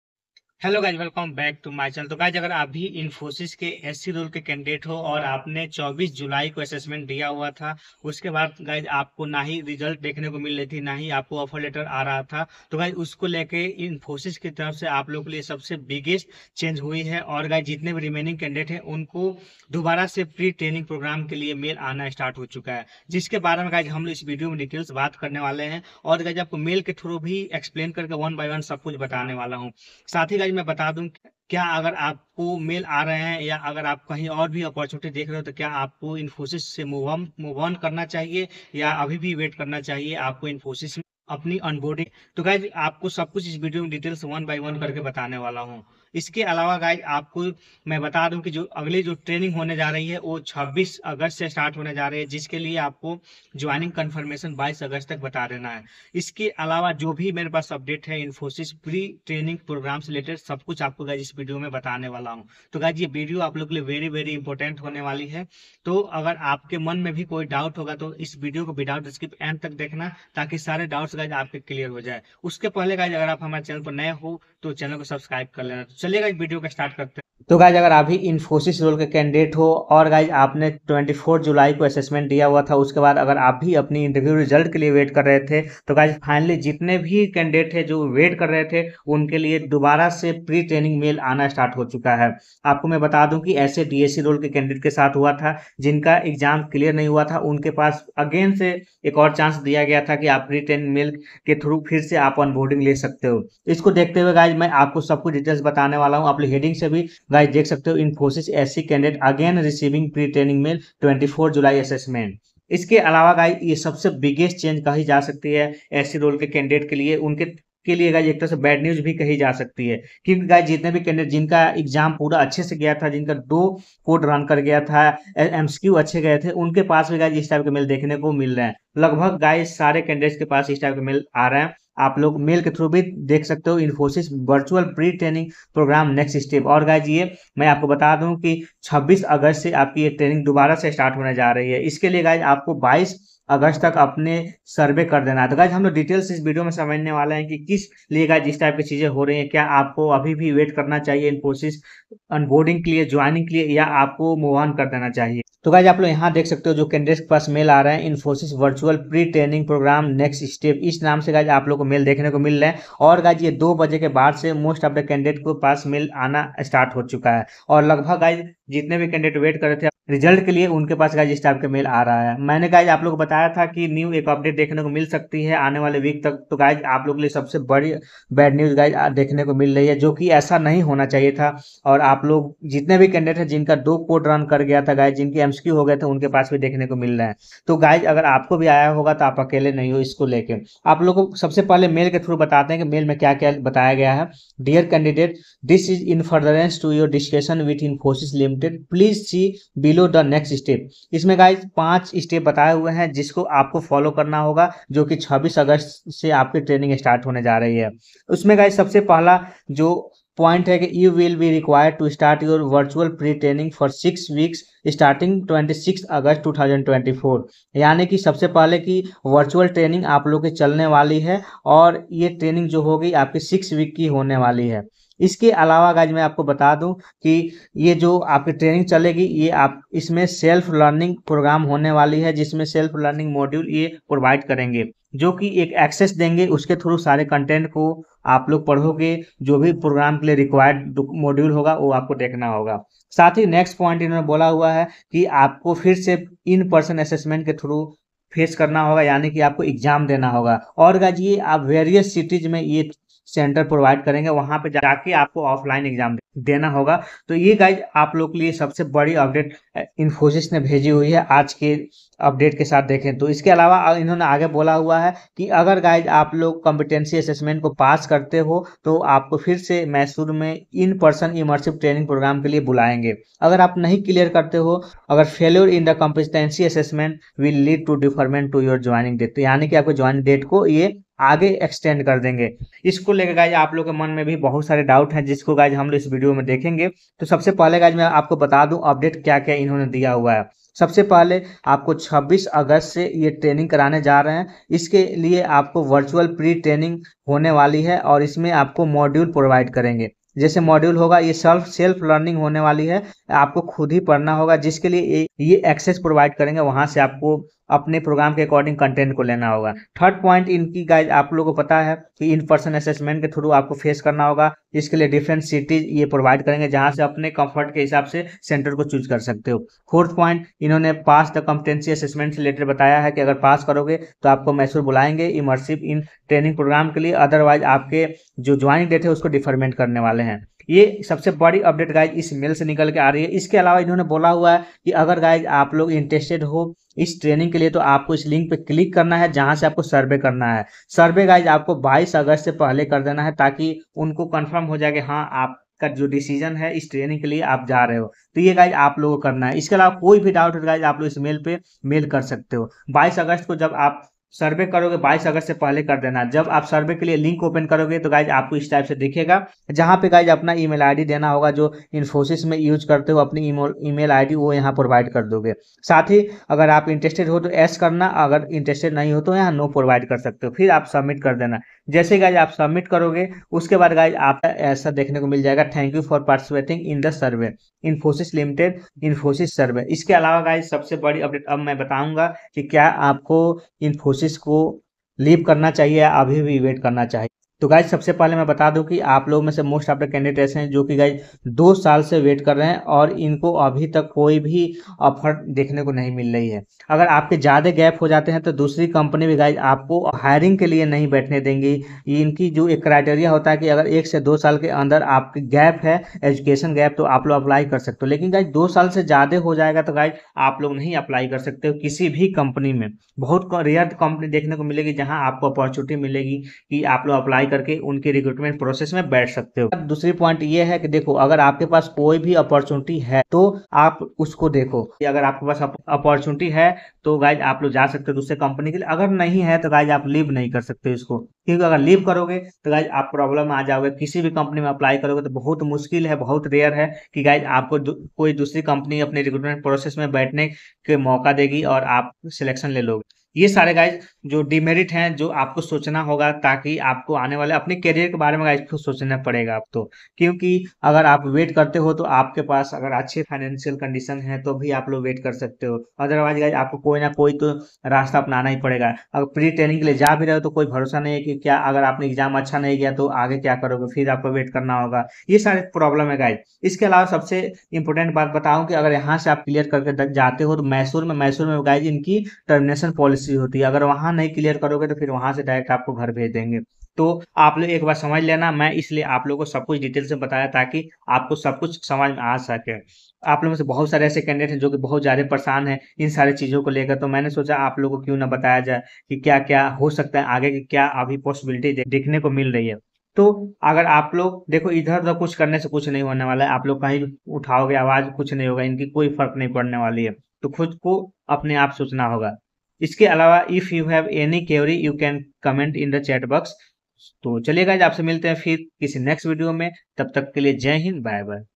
Infosys SE Candidates Again Pre-training Program 😱 || 6 Weeks Training || Self-study | 24 July Test
👉🏻 If you want any update on Joining/Onboarding or Hiring of the below mentioned companies, then you can join this Telegram channel.